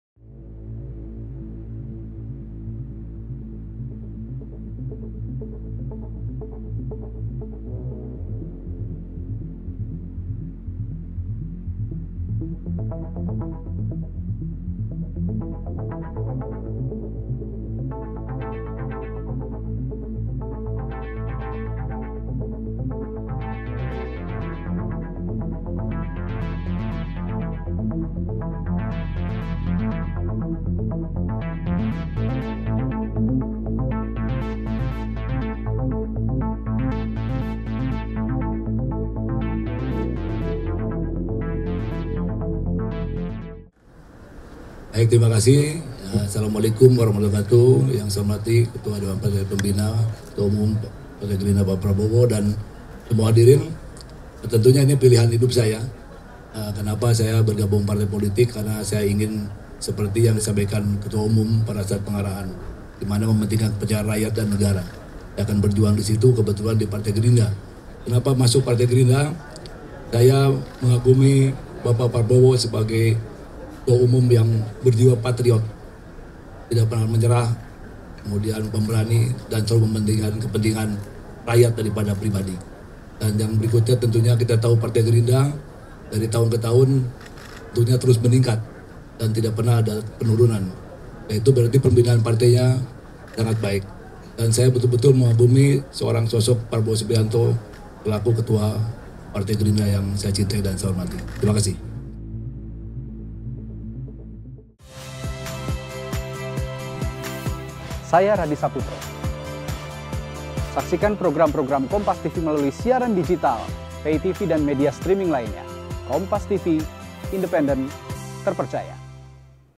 Iwan Bule Gabung Gerindra Karena Kagum dengan Sosok Prabowo
JAKARTA, KOMPAS.TV - Mochamad Iriawan alias Iwan Bule resmi bergabung ke Partai Gerindra. Iwan bule mengungkap alasan bergabung dengan Partai Gerindra. Ia mengaku bergabung ke Partai Gerindra karena mengagumi sosok Prabowo Subianto.

Hal ini disampaikan Iwan Bule dalam konferensi pers usai Rapat Koordinasi Partai Gerindra di kediaman Prabowo di Jalan Kertanegara, Jakarta Selatan, Kamis (27/4/2023).

"Kenapa masuk Gerindra? Saya mengagumi Bapak Prabowo sebagai Ketua Umum yang berjiwa patriot, tidak pantang menyerah, pemberani, dan selalu pentingkan kepentingan rakyat daripada pribadi," ujar Iwan Bule.